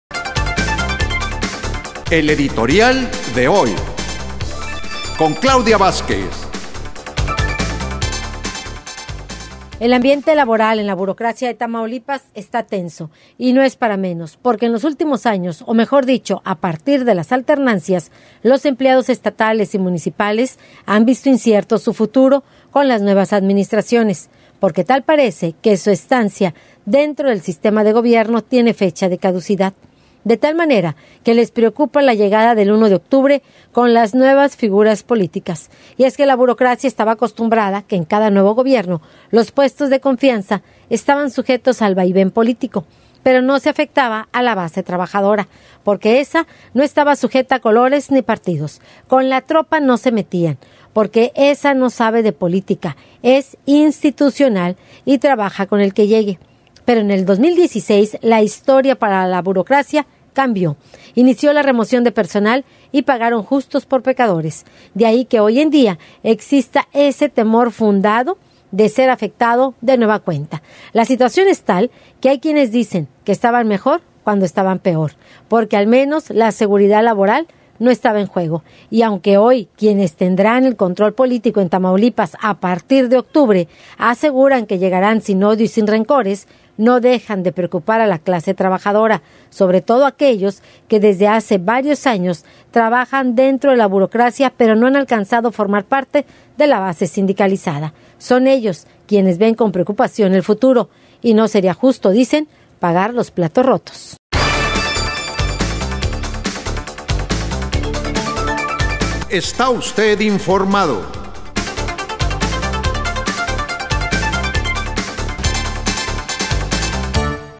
La burocracia está tensa
El editorial del día con Claudia Vázquez
ORT Noticias
Tamaulipas
15 junio del 2021